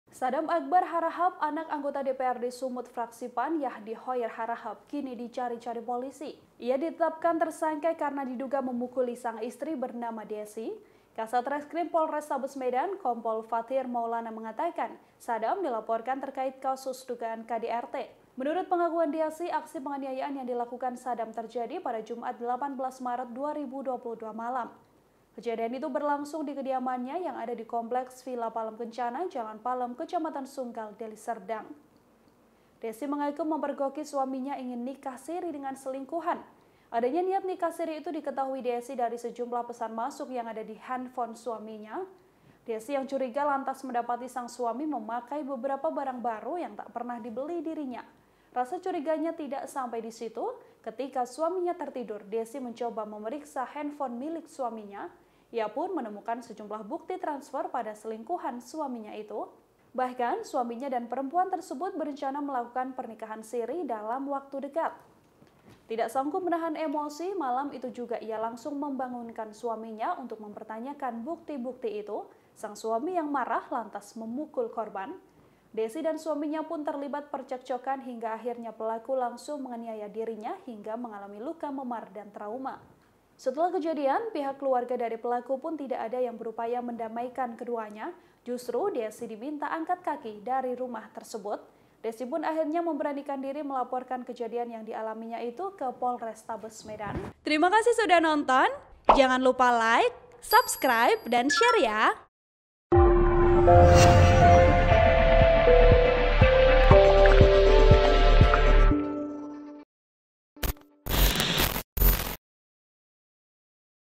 Pengakuan Istri Dianiaya Anak Anggota DPRD Sumut, Pergoki Suami Selingkuh Malah Diusir dari Rumah
TRIBUN-VIDEO.COM - Saddam Akbar Harahap, anak Anggota DPRD Sumut Fraksi PAN, Yahdi Khoir Harahap kini dicari-cari polisi.

Ia ditetapkan tersangka karena diduga memukuli sang istri bernama Deasy.

Kasat Reskrim Polrestabes Medan, Kompol Fathir Maulana mengatakan, Saddam dilaporkan terkait kasus dugaan KDRT.

Menurut pengakuan Deasy, aksi penganiayaan yang dilakukan Saddam terjadi pada Jumat (18/3/2022) malam.

Kejadian itu berlangsung di kediamannya yang ada di Komplek Villa Palem Kencana, Jalan Palem, Kecamatan Sunggal Deli Serdang.

Deasy mengaku memergoki suaminya ingin nikah siri dengan selingkuhan.

Adanya niat nikah siri itu diketahui Deasy dari sejumlah pesan masuk yang ada di handphone suaminya.

Deasy yang curiga lantas mendapati sang suami memakai beberapa barang baru yang tak pernah dibeli dirinya.

Rasa curiga nya tidak sampai di situ, ketika suaminya tertidur Deasy mencoba memeriksa handphone milik suaminya.

Ia pun menemukan sejumlah bukti transfer kepada selingkuhan suaminya itu.

Bahkan, suaminya dan perempuan tersebut berencana melakukan pernikahan siri dalam waktu dekat.

Tidak sanggup menahan emosi, malam itu juga ia langsung membangunkan suaminya dan mempertanyakan bukti - bukti itu.

Sang suami yang marah lantas memukul korban.

Deasy dan suaminya pun terlibat percekcokan hingga akhirnya pelaku langsung menganiaya dirinya hingga mengalami luka memar dan trauma.

Setelah kejadian, pihak keluarga dari pelaku pun tidak ada yang berupaya mendamaikan keduanya.

Justru, Deasy diminta angkat kaki dari rumah tersebut.

Deasy pun akhirnya memberanikan diri melaporkan kejadian yang dialaminya itu ke Polrestabes Medan.

(Tribun-Video.com/TribunMedan)